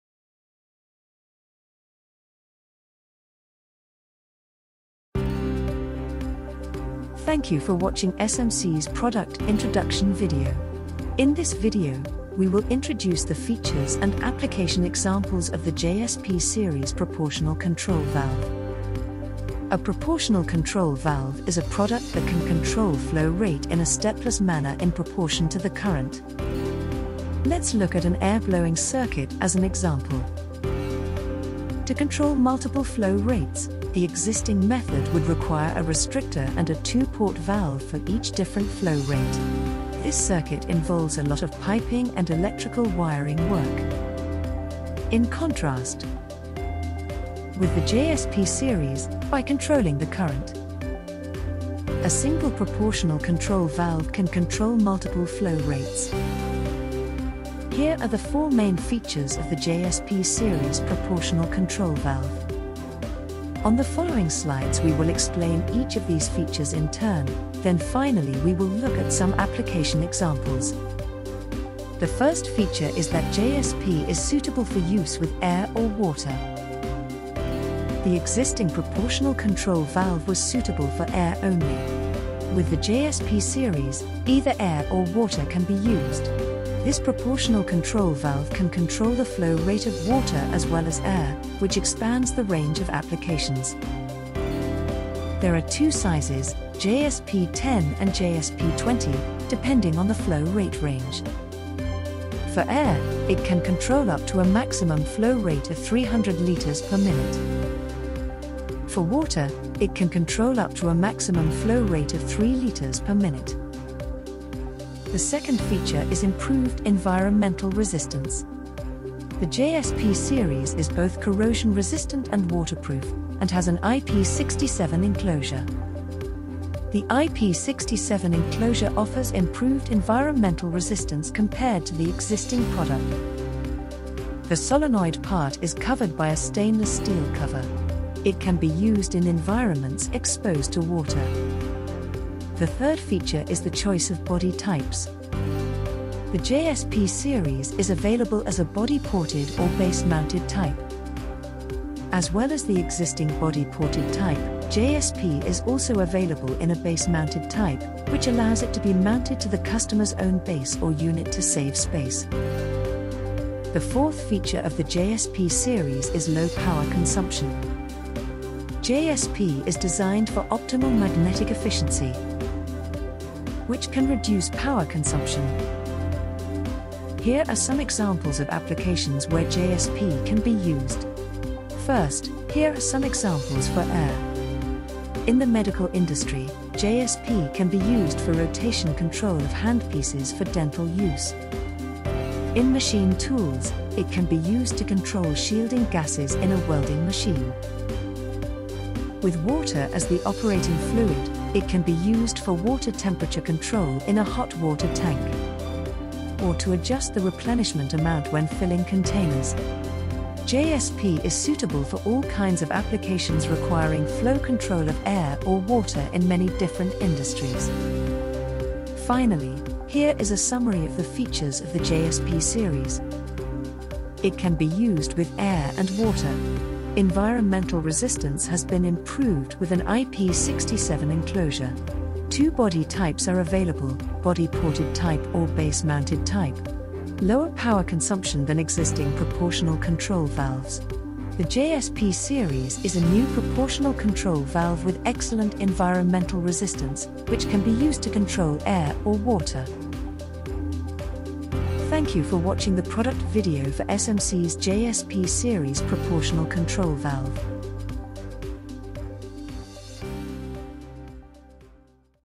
SMC proportional control valve JSP series
This video will show you the features and applications of SMC's proportional control valve JSP series.

・Control the flow rate smoothly according to the current
・Repeatability: ±3% F.S. or less
・Sensitivity: 2% F.S. or less
・2 body types: Body ported, Base mounted
・IP67 enclosure
・Seal material: FKM
・Choice of body material: Stainless steel, Brass
・Electrical entry: Grommet, Conduit, DIN terminal

If the content is useful, please hit like to support us.